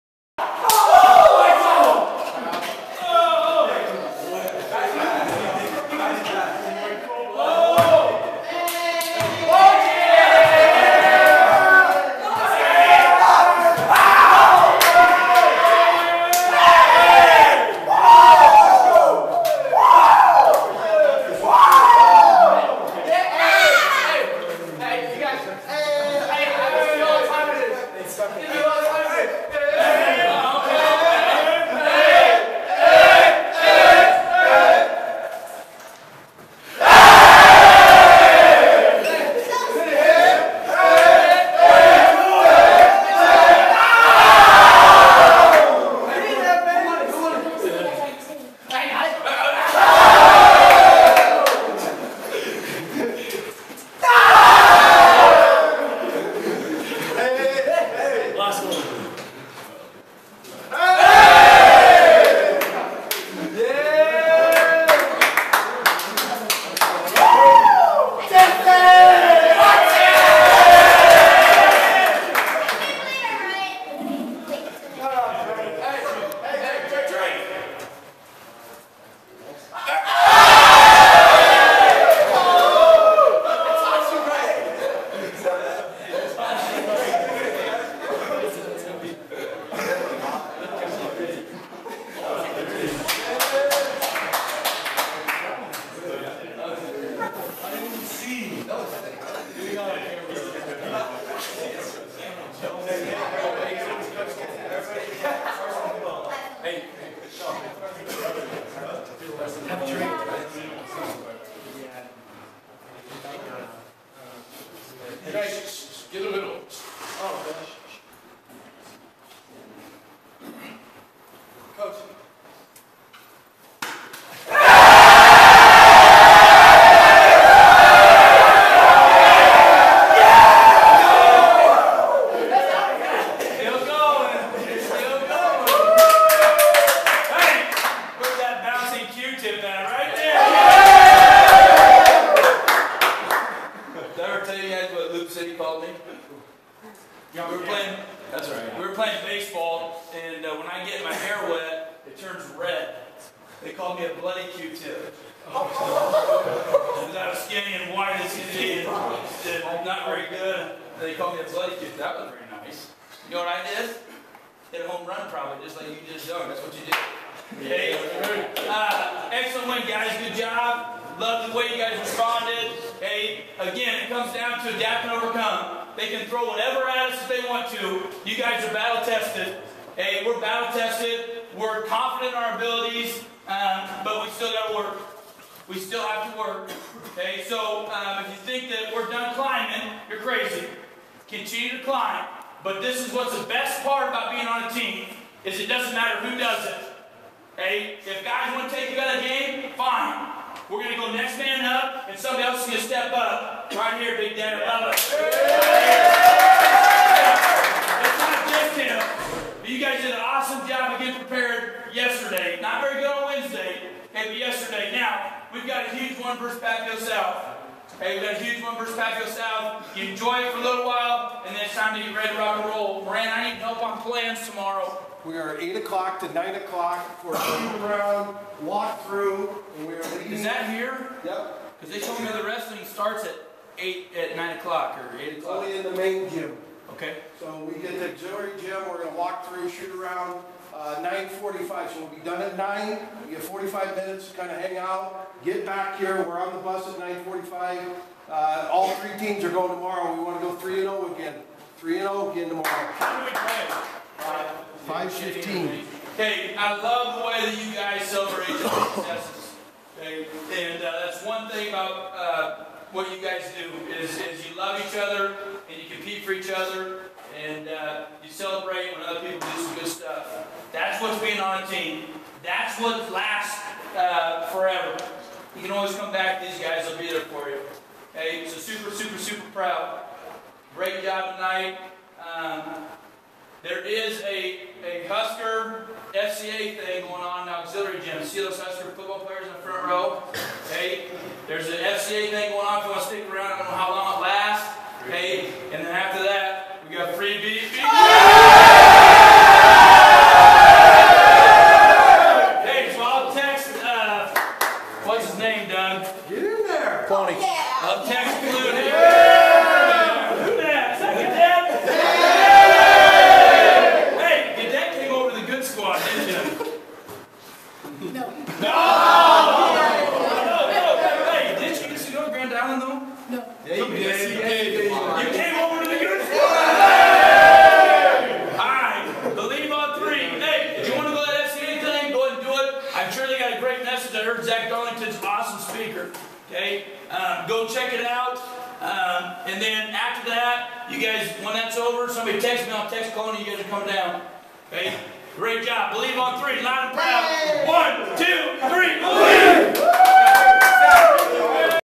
Kearney High Locker Room Celebration after WOGI -- Next Man Up!
Go inside the Kearney High basketball locker room for the celebration & Coach Beranek's comments after KHS wins 11th straight game, beating Grand Island 55-41.